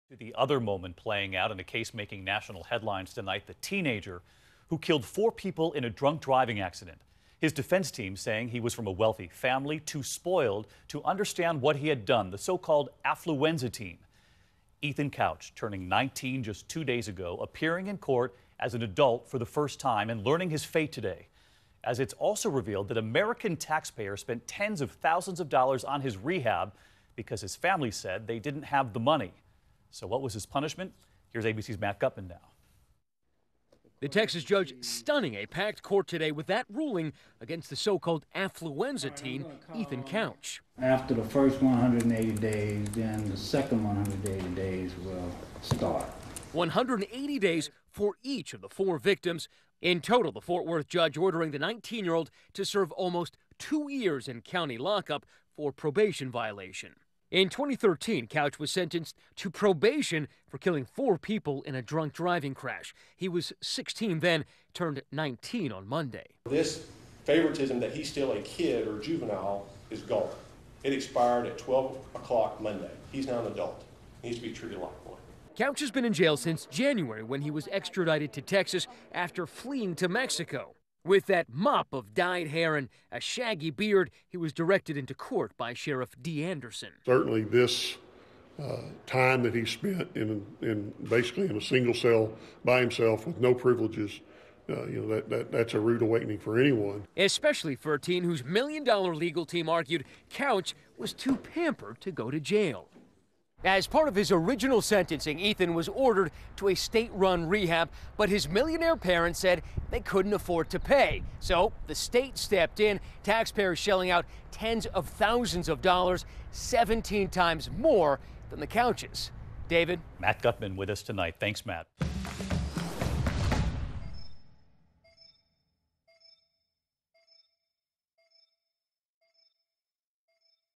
'Affluenza' Teen Ethan Couch to Serve Two-Year Sentence
'Affluenza' Teen Ethan Couch | 19-year-old to serve almost a two-year sentence for probation violation.